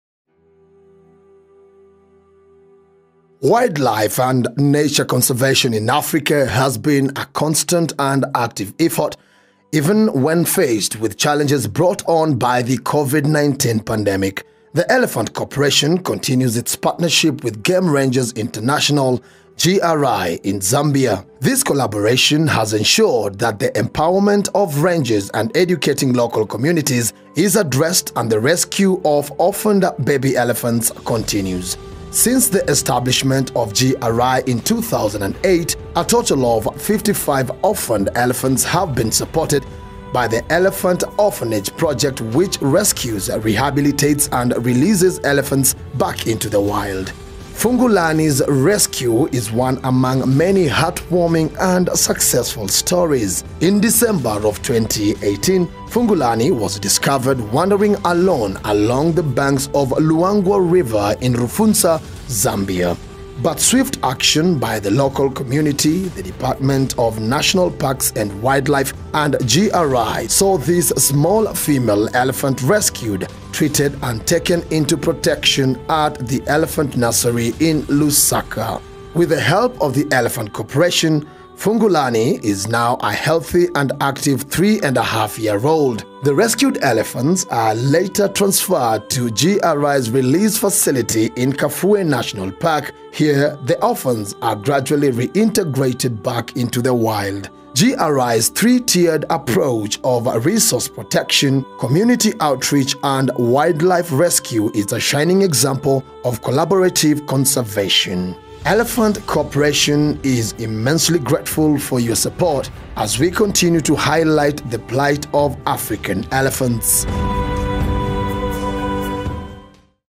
rElephant Report GRI
Wildlife and nature conservation in Africa has been a constant and active effort, even when faced with challenges brought on by the COVID-19 pandemic. We are so proud to support Game Rangers International (GRI) in Zambia to ensure that the empowerment of rangers and educating local communities is addressed and the rescue of orphaned baby elephants continues.

Narrator: Patrick Igunza